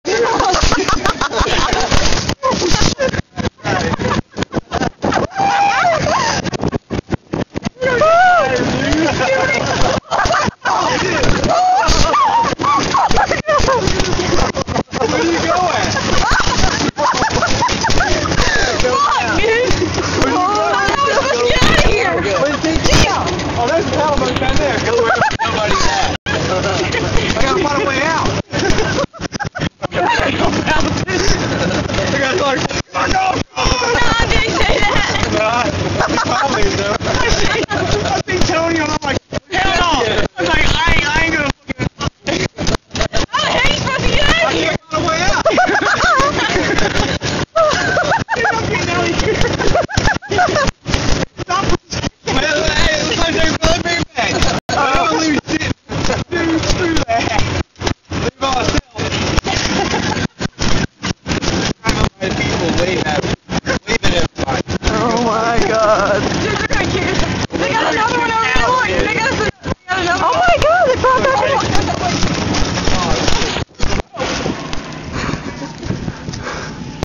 Golf Cart Steal @ Company Picnic
My friend stole a golf cart at a company outing, he was kicked out after they caught him!